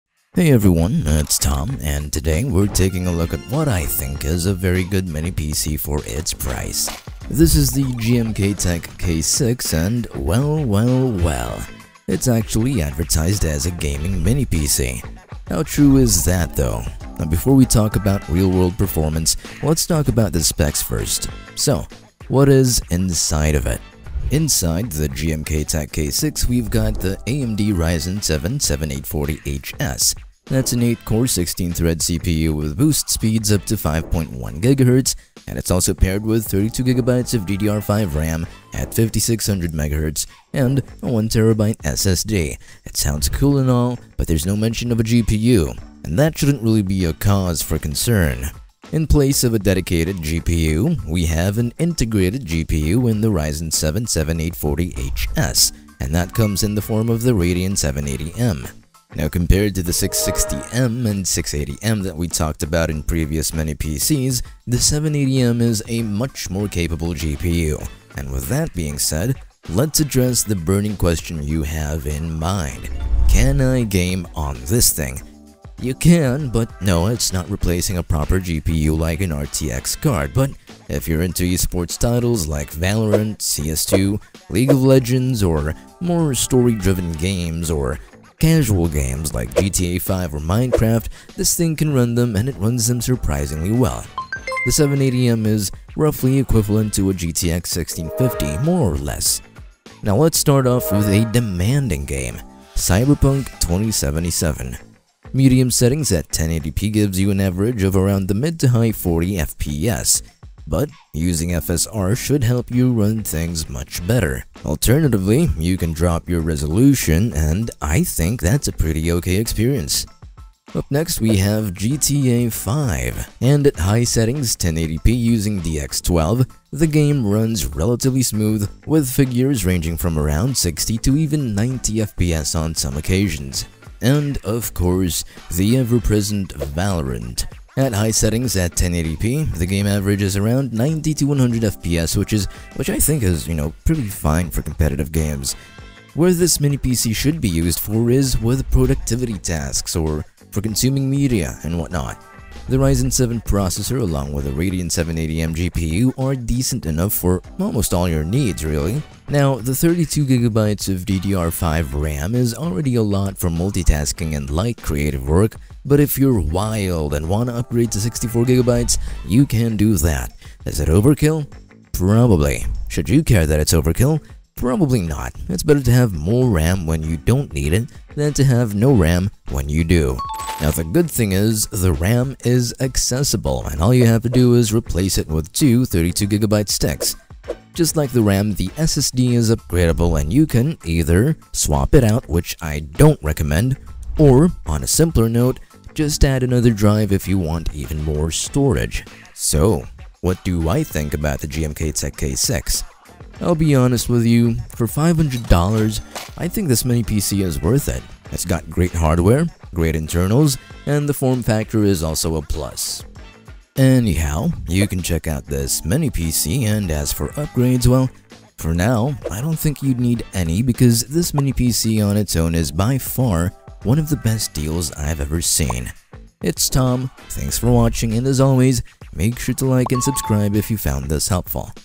The BEST Mini PC under $500! – GMKtec K6 Mini PC Gaming
Are you looking for the best Mini PC under 500 Dollars? Then you've come to the right place! In this video I'll show you everything you need to know about the GMKtec K6 Mini PC Gaming.

*Compare prices now (International Link):*
👉 GMktec K6